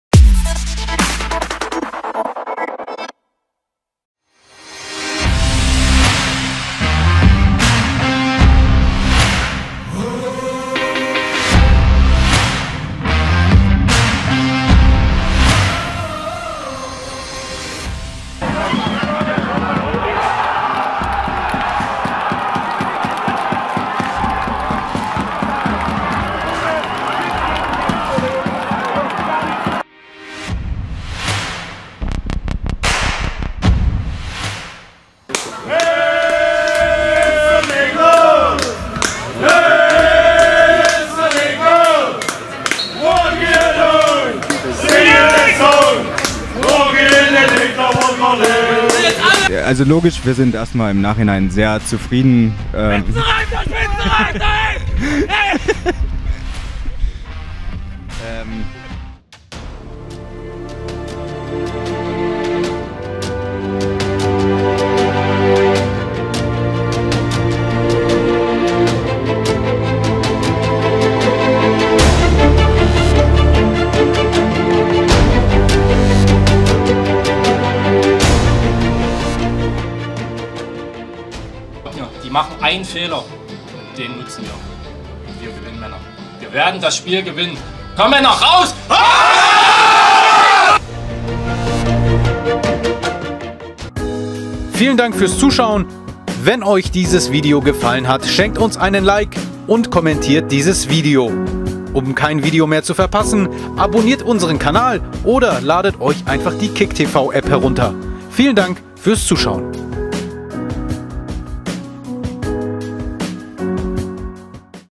Die SACHSENKICK.TV Highlights 2019
Emotionale Momente, ausgefallene Jubel und spannende Geschichten.
2019 war ein tolles Fußballjahr! Wir haben euch unsere Highlights zusammengeschnitten. Viel Spaß!